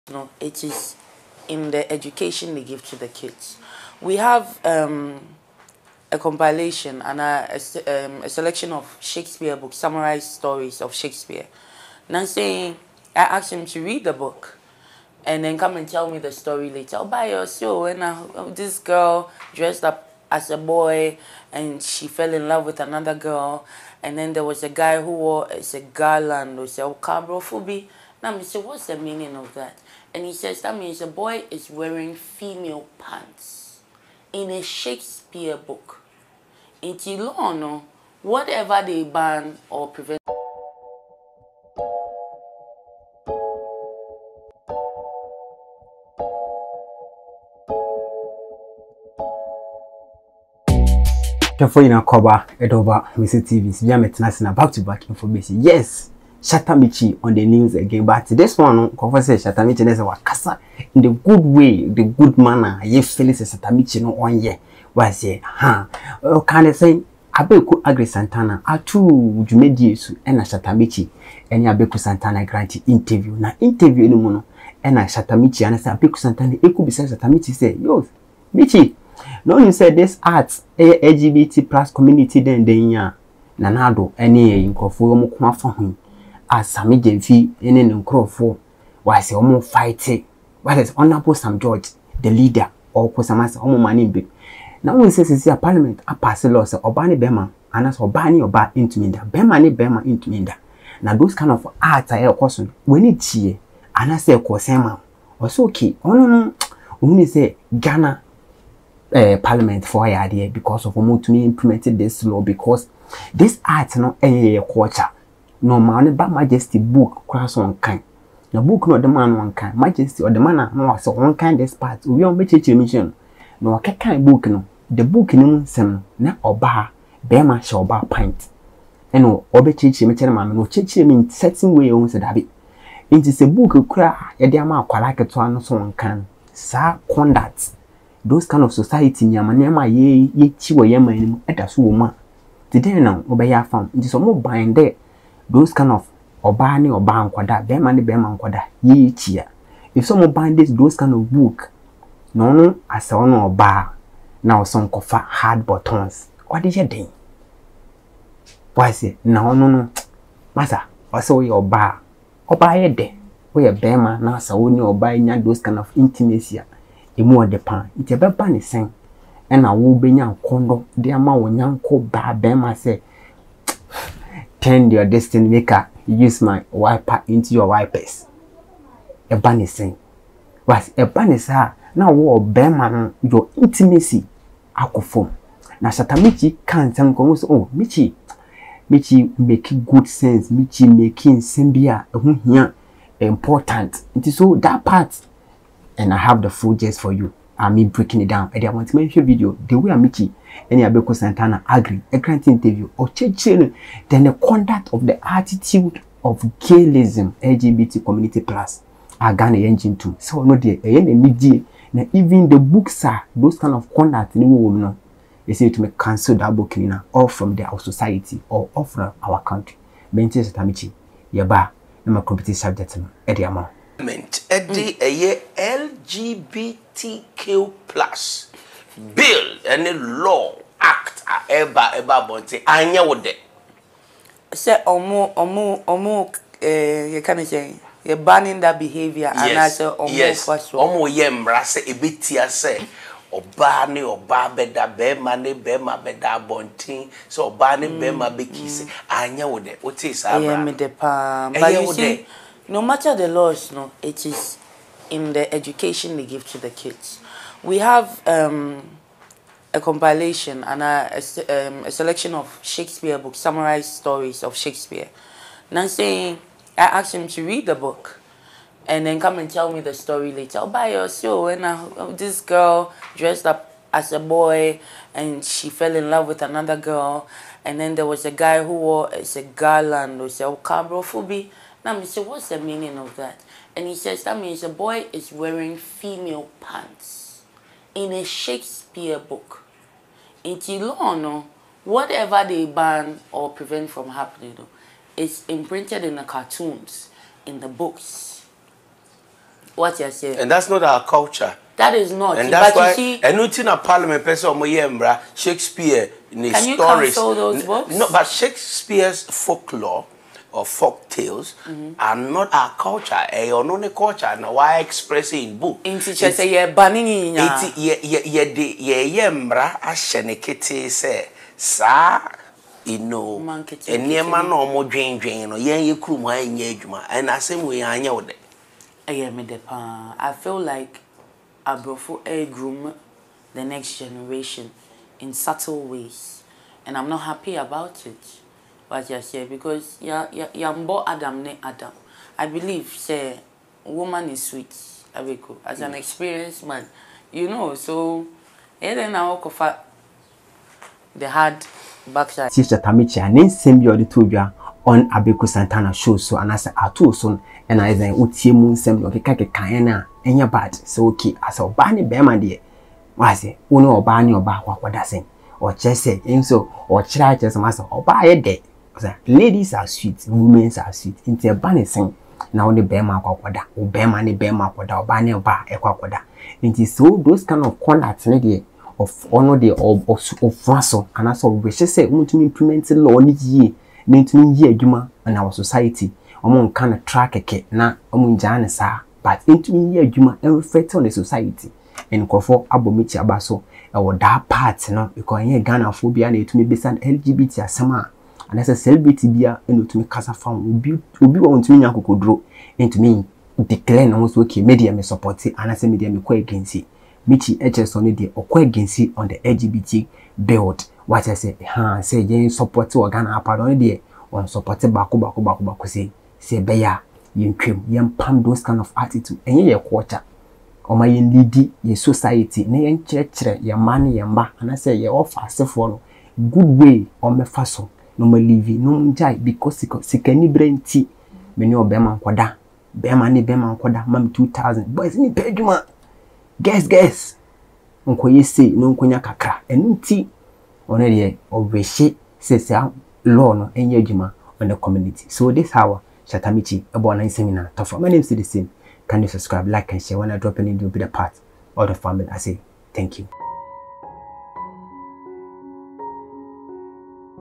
OMG😳 Woman is Sweet, So Why Do You Go For The Hard Backside G@y Or LGBTQI Shatta Michy
AMISTY TV